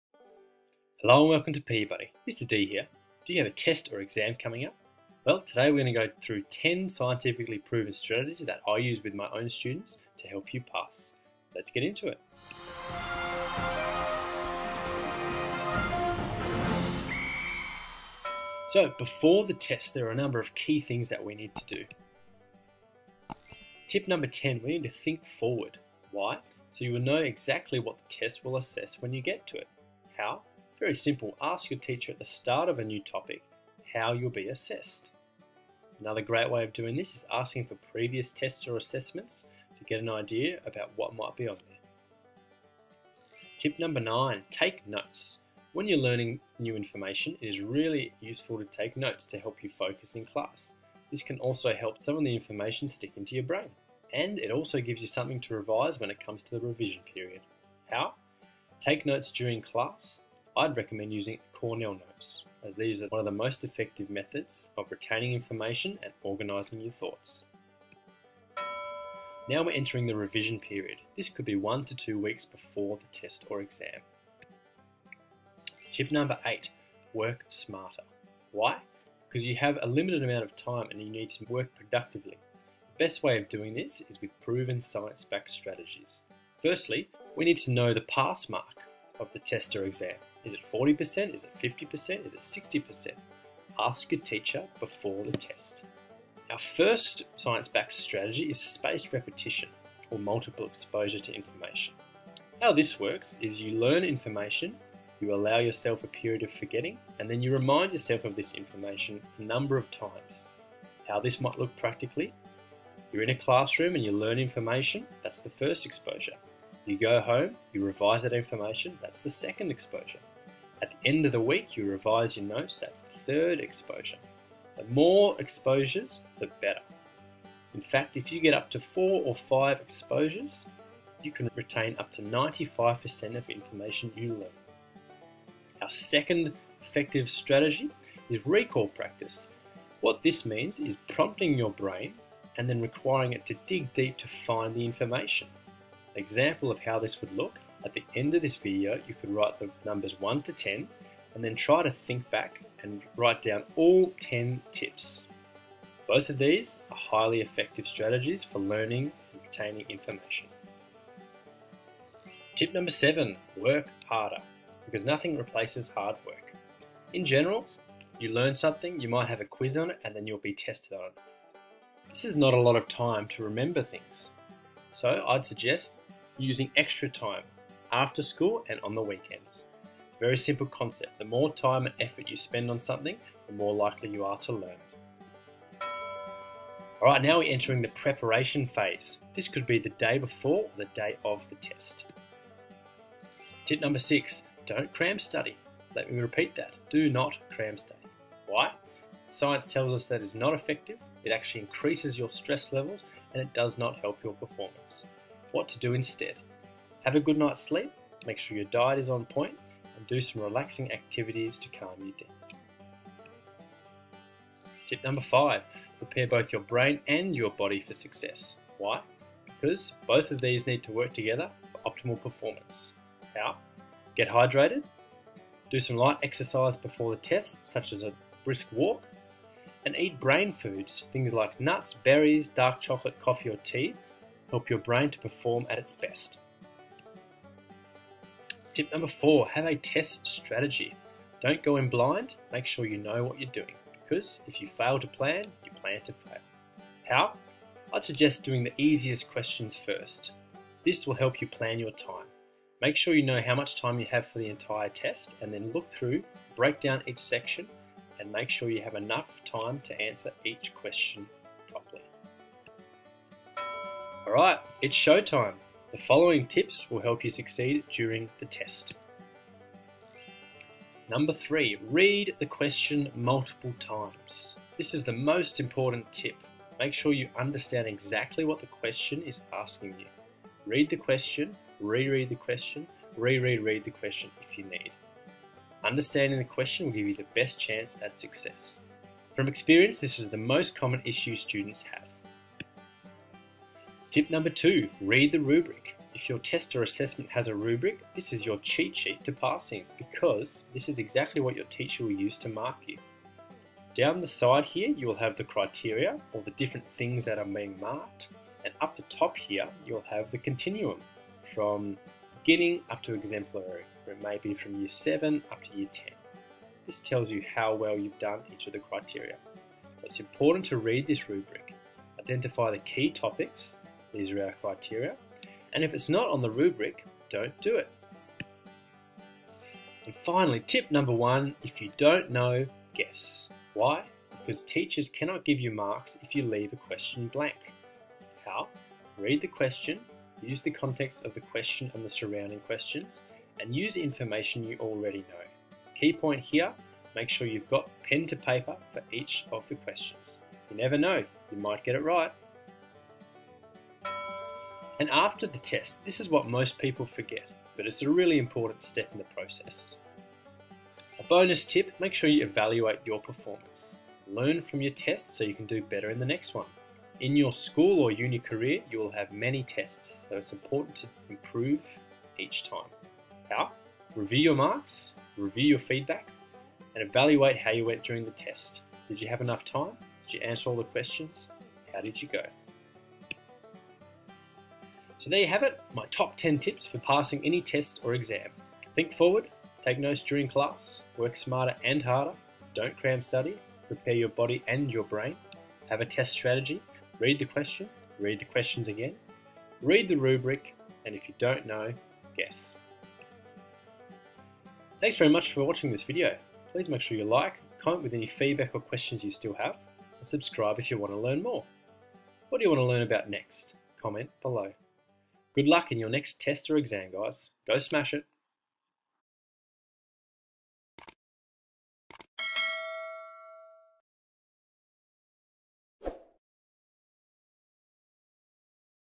Exam Success Strategies No One is Telling You!
Hello and welcome to PE Buddy, Mr D here!

Do you have a test or exam coming up? In today's video you'll learn how to pass any test or exam with 10 science-backed tips from a teacher!

0:00 Introduction and overview
0:18 Before the test/exam
1:08 Revision phase (1-2 weeks before the test/exam)
3:05 Preparation phase (The day before or day of the test/exam)
4:23 During the test! Tips, tricks and strategies
5:53 After the test (The phase most people forget!)
6:24 Summary of strategies

We'll cover science-backed learning methods, preparation tips, best ways to study, mid-test strategies and nutrition hacks! Don't spend hours endlessly revising inefficiently. Make your study and revision efficient by using proven strategies.

This video will cover the learning phase, preparation phase, during the test and post-test phase!

Give these strategies a go on your next test and please let me know how you go!

Mr D